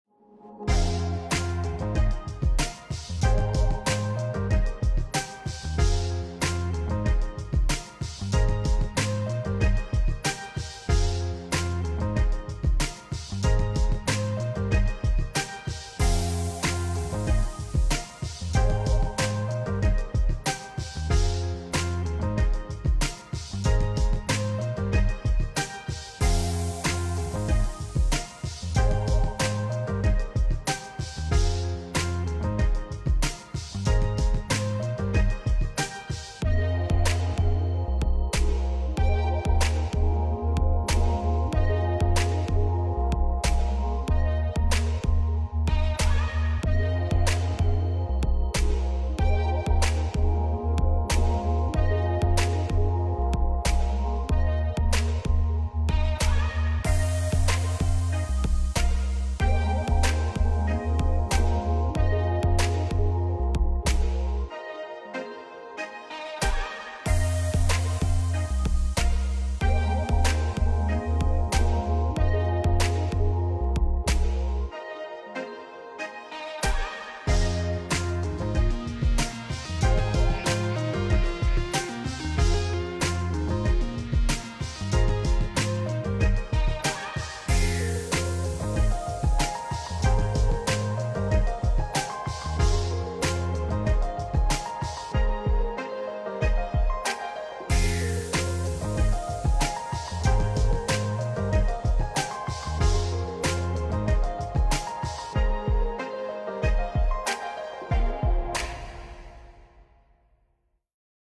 Drawing Zeema From Animal Wonders Montana!
Drawing Zeema From Animal Wonders Montana! Hey everyone, today we draw Zeema the Keel-billed toucan from Animal Wonders! I love their work, saving rejected exotic animals and giving them an amazing life!